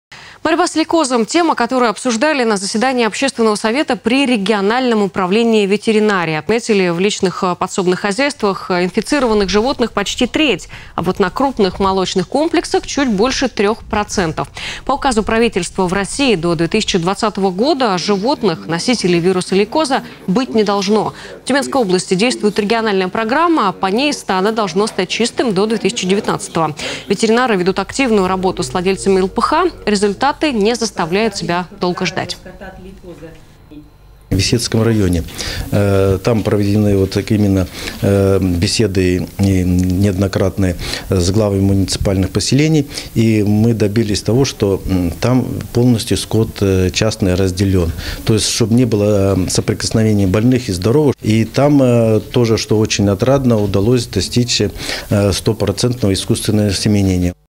Почти треть животных в личных подсобных хозяйствах инфицированы лейкозом
Борьба с лейкозом - тема, которую обсуждали на заседании общественного совета при региональном управлении ветеринарии Отметили, в личных подсобных хозяйствах инфицированных животных почти треть. А вот на крупных молочных комплексах - чуть больше трех процентов. В России до 2020 года животных - носителей вируса лейкоза быть не должно. В Тюменской области действует региональная программа. По ней стадо должно стать чистым до 2019-го. Ветеринары ведут активную работу с владельцами ЛПХ. Результаты не заставляют себя долго ждать.